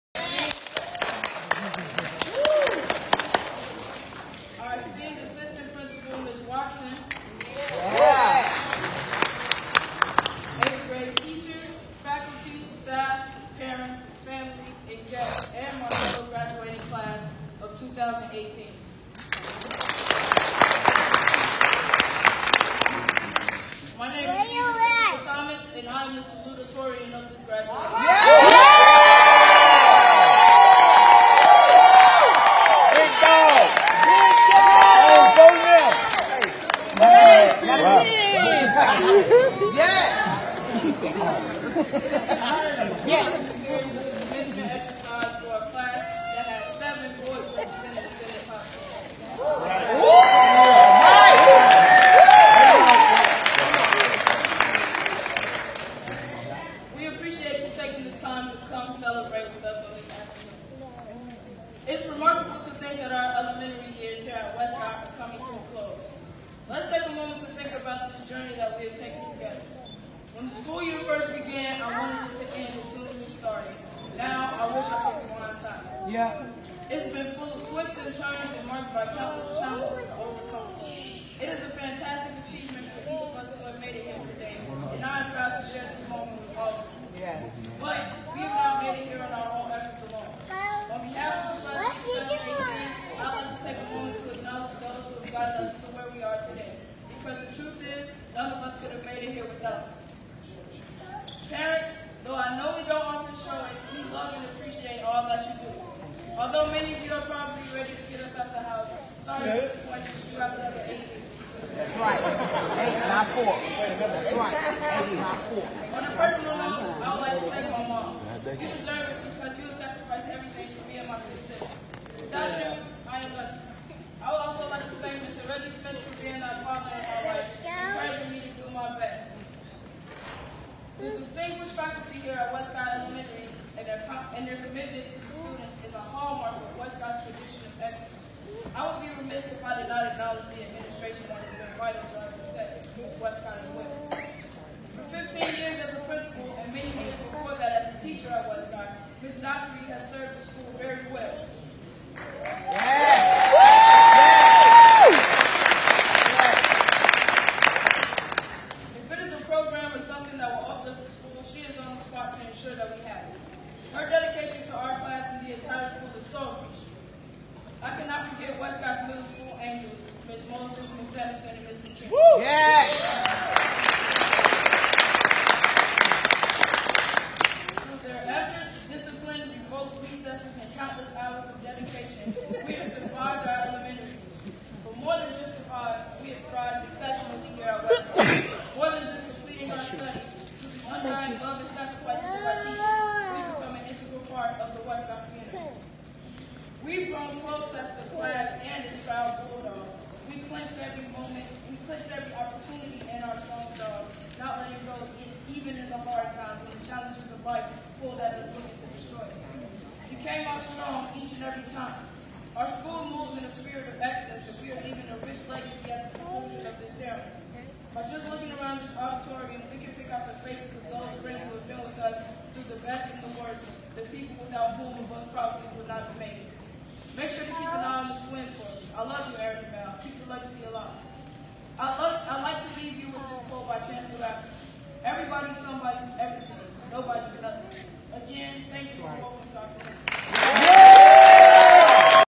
Steve's 8th Grade Graduation
Stephen aka Steve Thomas was Salutatorian of his 8th-grade graduation CLASS OF 2018! He is a student at De La Salle Institute. He is also one of two freshmen who was admitted to the Kappa League Chicago this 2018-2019 school year in addition to being a LINK Unlimited Scholar.

Upon graduation from high school, Steve's dream and aspiration are to become a Marine Biologist.

Steve has participated in the Chicago Metro History Fair 3 years consecutively earning three trips to State Finals and one of two exhibits in both the Junior and Senior categories to represent the State of Illinois.

In elementary school, Steve represented his school, Oliver S. Westcott Elementary School at the Chicago Public Schools Citywide Spelling Bee three years consecutively having placed in the top five his 7th-grade year and the top 3 his 8th-grade year.

Upon graduation from elementary school, he earned several monetary awards and scholarships from Excellence in Academics and was recognized by both Omega Psi Phi Fraternity, Inc. and Alpha Phi Alpha Fraternity Inc. for his Academic Excellence.

Steve's essay at De La Salle Institute was selected among 3K in a National Writing Contest for which he’ll be recognized on May 11, 2019. His current project will be displayed on May 21, 2019, at the Chicago History Museum.